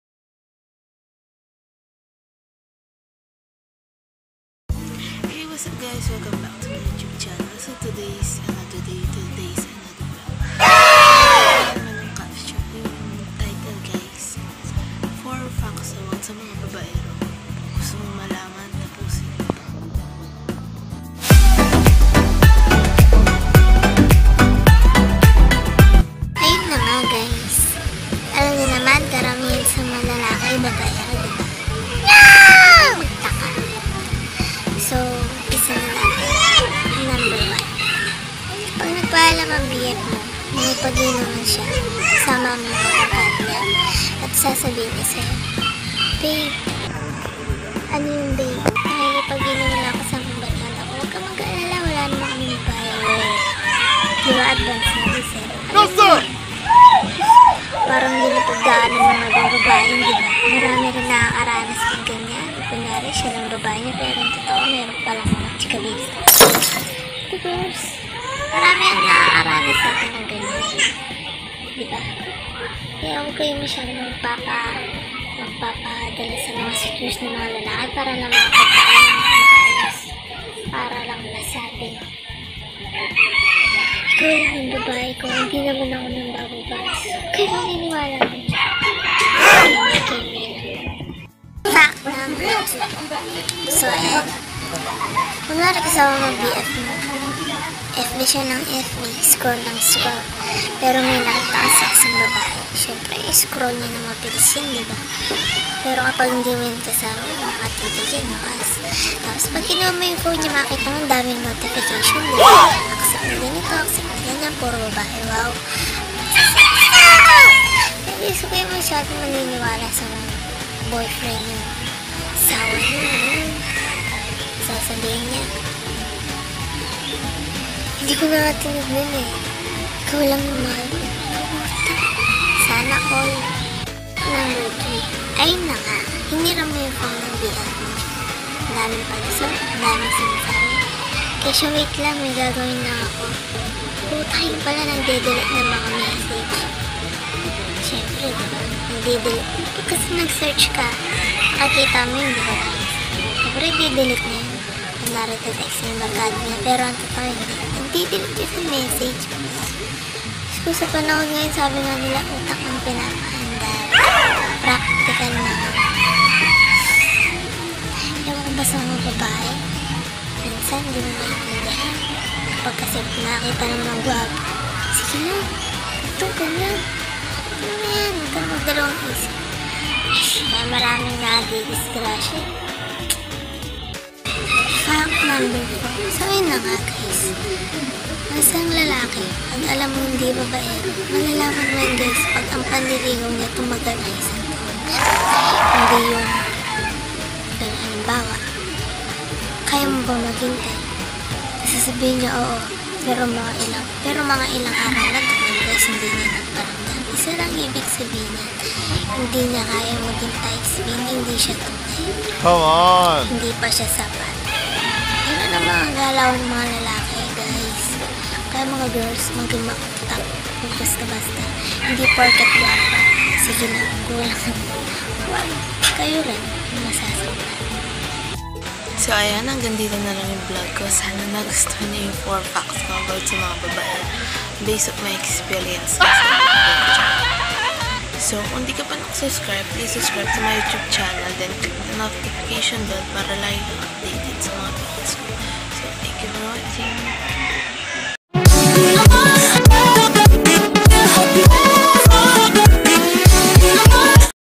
4 FACTS ABOUT SA MGA BABAERO | BASE ON MY EXPERIENCE 🤣
SHARE KO LANG YUNG OPINION KO GUYS, SANA NAGETS NYO DAMI KASI AKONG KAKILALA NA GANYAN SILA HAHAHAHAHA.😅